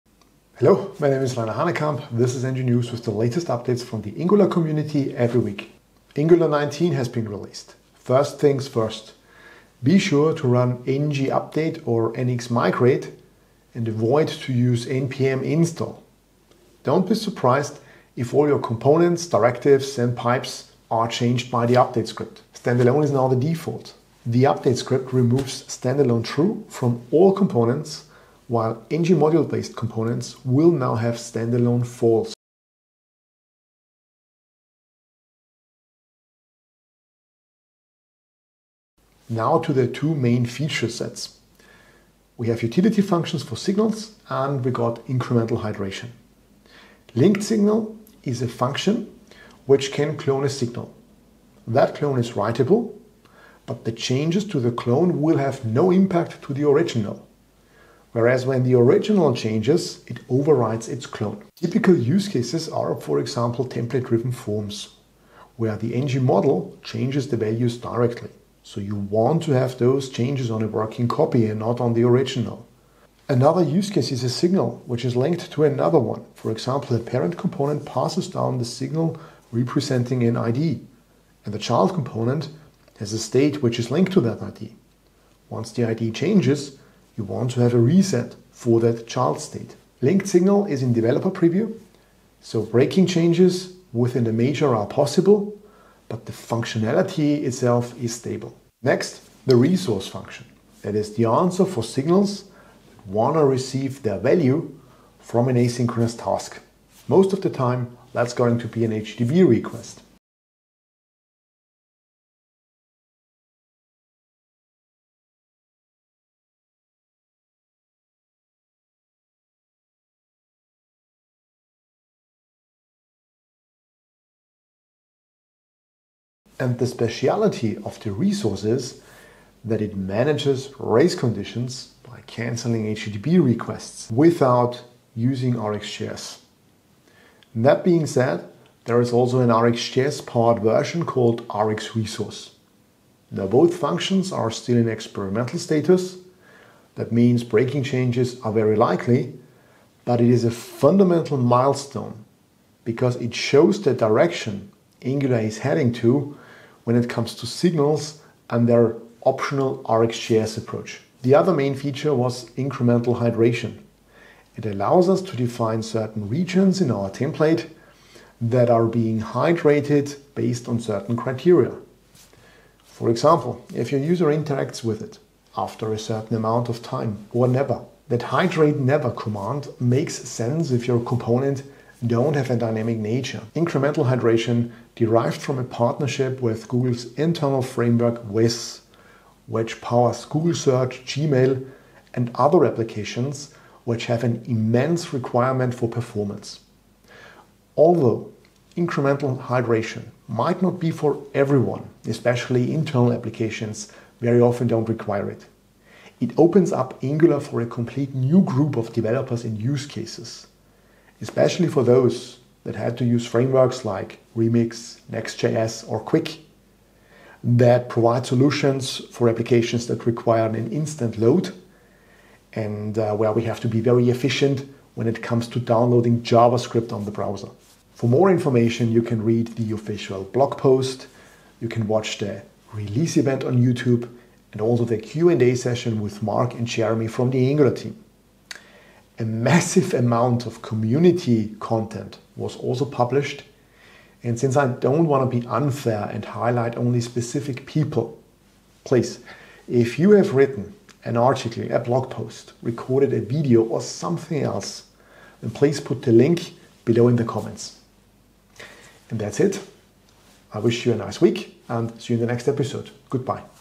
Episode 24/47: Angular 19
Angular 19 has been released!

## Standalone by default

First things first—when you update, make sure to run `ng update` or `nx migrate`, but avoid using the native `npm install` command.

Don’t be surprised if the update script changes all your components. **Standalone is now the default**.

The update script removes `standalone: true` from all components, while NgModule-based components will now have `standalone: false` added.

## Utility Functions for Signals

### `linkedSignal()`

With `linkedSignal`, you can clone an existing Signal. You can modify the clone, but those changes won’t affect the original. However, if the original Signal updates, its value will overwrite the clone.

This is perfect for:

1. Template-driven forms, where you need a working copy of a Signal.

2. Managing a state linked to a parent Signal. For example, when a parent component passes down an ID to a child component, and the child’s state needs to reset when the parent’s Signal changes.

`linkedSignal` is in **developer preview**. While the functionality is stable, breaking changes within a major version are still possible.

### `resource()` & `rxResource()`

The `resource` function addresses Signals where the value comes from an asynchronous task, such as an HTTP request. Its standout feature is managing race conditions by automatically canceling HTTP requests — without using RxJS.

Additionally, there’s an RxJS-powered version called `rxResource`.

Both functions are still **experimental**, so breaking changes are likely.

However, this marks a fundamental milestone, showcasing Angular’s direction with Signals and its "optional RxJS" approach.

## Incremental Hydration

Incremental Hydration allows parts of your template to hydrate based on specific triggers.

For example:
- When a user interacts with it.
- After a delay.
- Or never

This feature comes from Angular’s collaboration with Wiz, Google’s internal framework used in Gmail, Google Search, and other performance-intensive applications.

While Incremental Hydration might not be relevant for all use cases — such as internal applications — it opens Angular to a completely new group of developers.

These are developers who previously relied on frameworks like Next.js, Remix, or Qwik to build interactive websites that load instantly and are efficient in terms of bundle size.

The official release video on YouTube.

The Q&A session with Mark and Jeremy from the Angular team.

The Angular blog post